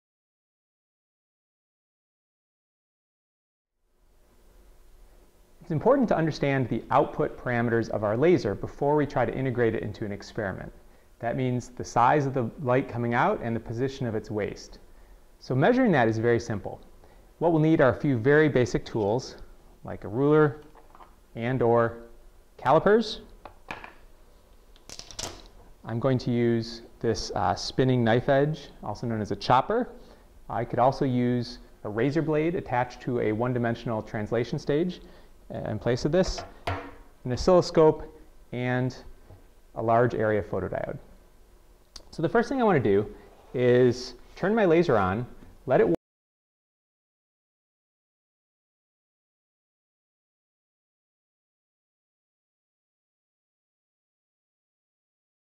Measuring Gaussian Beam Parameters SAMPLE
A look at how to measure the Gaussian beam parameters for a laser beam using a chopper and an oscilloscope.  This is a video sample from the eBook "Laboratory Optics: A practical guide to working in a laser optics lab".